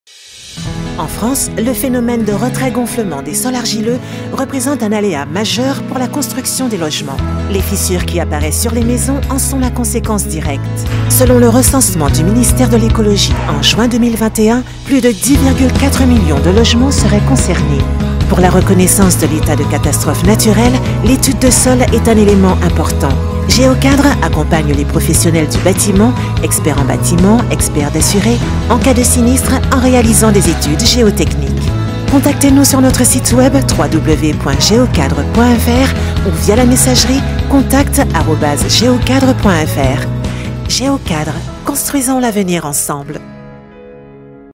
Géocadre - etude de sol - Retrait gonflement sol argileux - geotechnique
La France est de plus en plus frappée par des phénomènes climatiques sévères.

Ces phénomènes climatiques sont la principale cause du déclenchement des aléas retrait- gonflement des sols argileux.

En 2020, les dédommagements de sinistres (sur les bâtiments) liés au phénomène de retrait-gonflement se chiffrent à environ 1 Milliard d'Euros.

Geocadre vous accompagne dans l'étude de sol avant la construction ou en cas de sinistre.